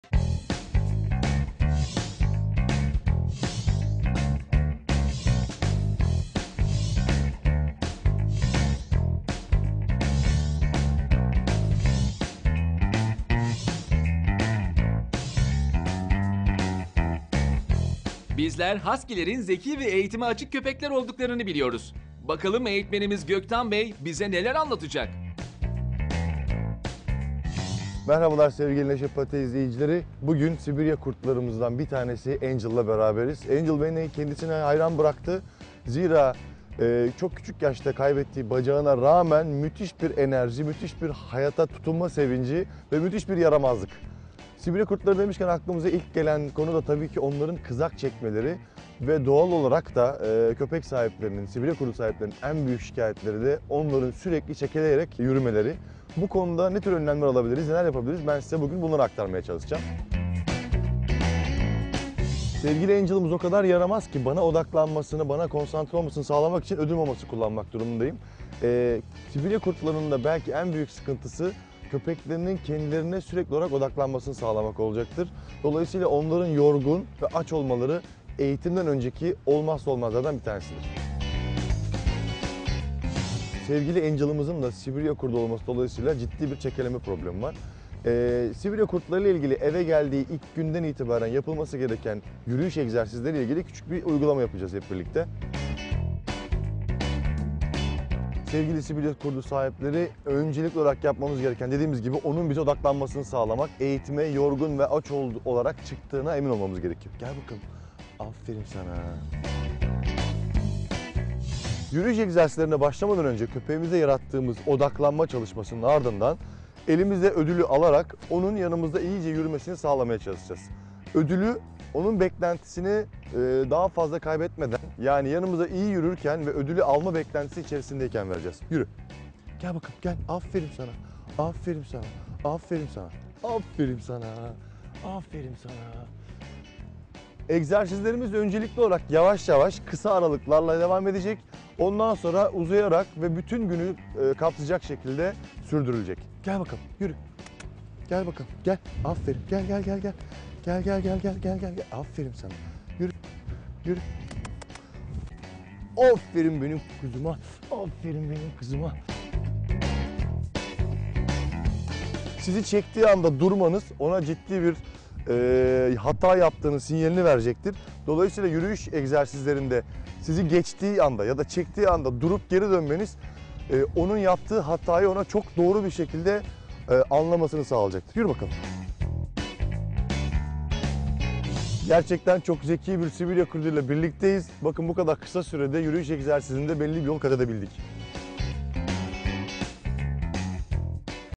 Husky'ler - Ev ve iş köpekleri eğitimi
"NEŞELİ PATİLER"de bu haftanın kahramanı Sibirya kurdu olarak da bilinen "Husky"ler..

Köpek Eğitim Uzmanı Göktan Eker, Husky'lerin bakımı, eğitimi ve özellikleri hakkında bilgi veriyor.